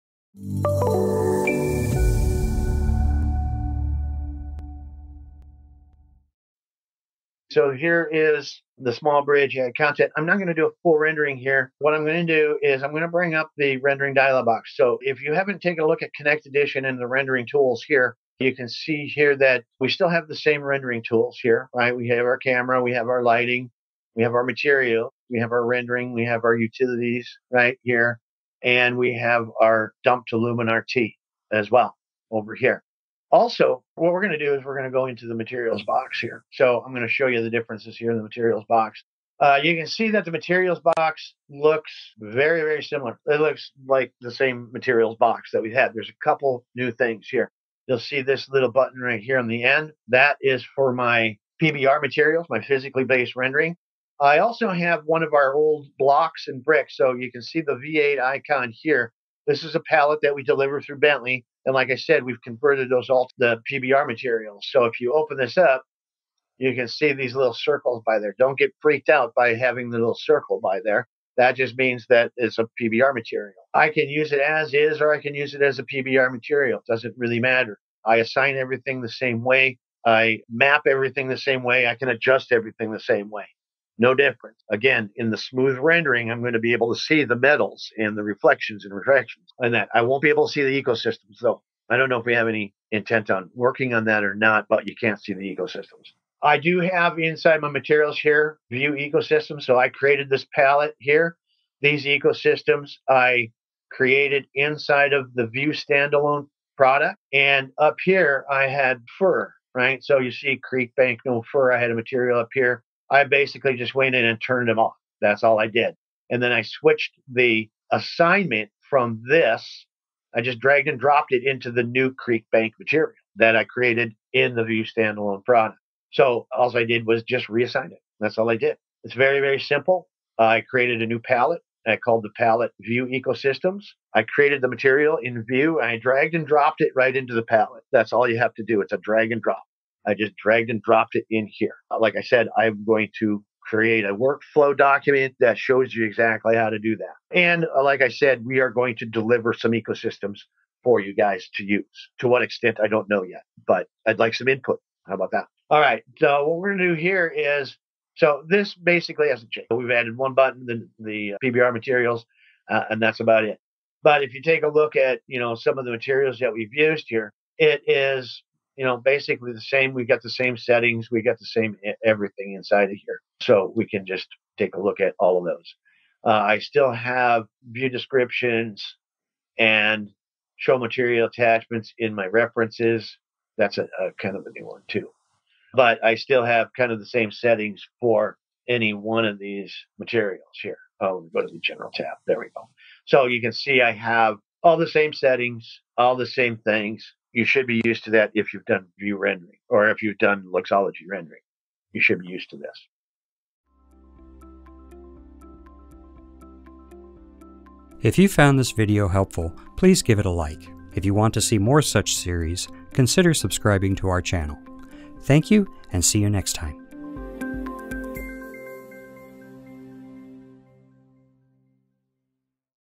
Material Editor
Hear from Steve Rick, Senior Consultant, and learn more about rendering and visualization, and the latest MicroStation update.